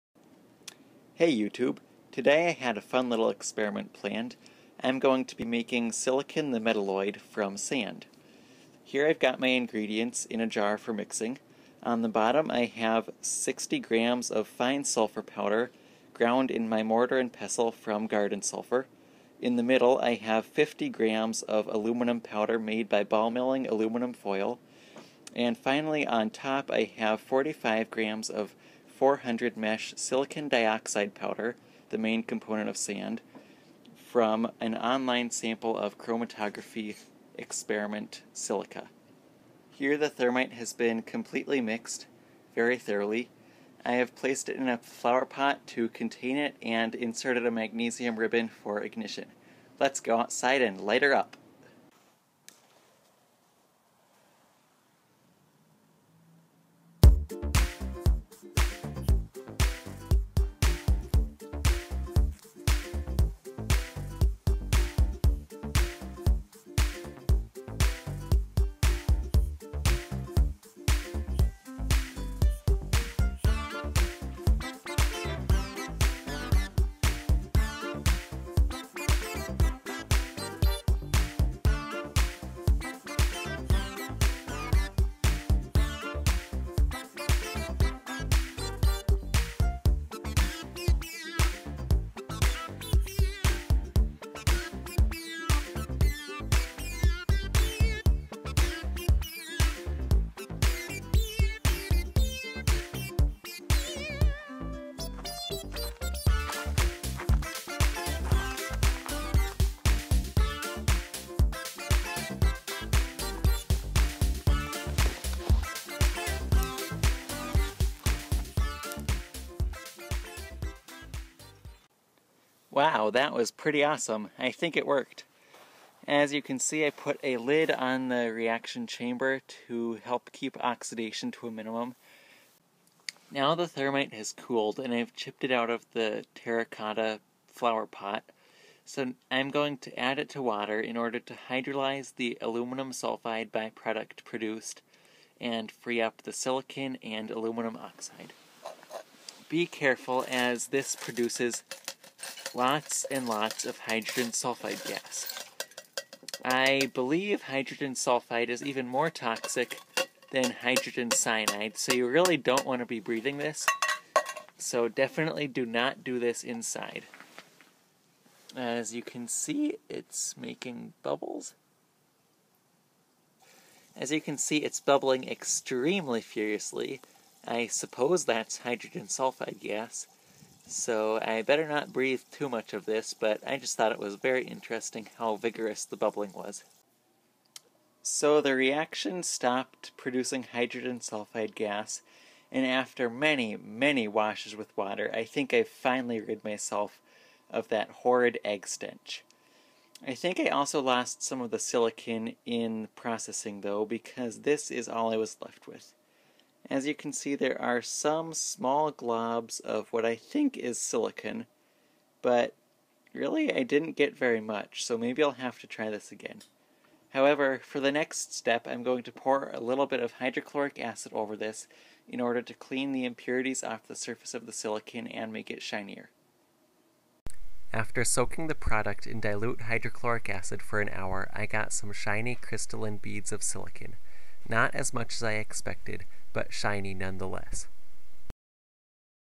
Silicon - Summer of Thermite
This is a 12:10:9 mass ratio of powdered sulfur to 400 mesh silicon dioxide to ball-milled aluminum.  I mixed it well and lit it using a magnesium fuse.  Dumping the cooled slag into water destroyed the aluminum sulfide but made hydrogen sulfide, a toxic and stinky gas.  Take care with this poison!

Once I sifted the silicon beads from the slag, I soaked them for an hour in dilute hydrochloric acid to clean them up.  I got less than I had hoped for, but they were shiny and showed some neat crystals.  The flame of the thermite was impressive, so I was happy with how the experiment turned out.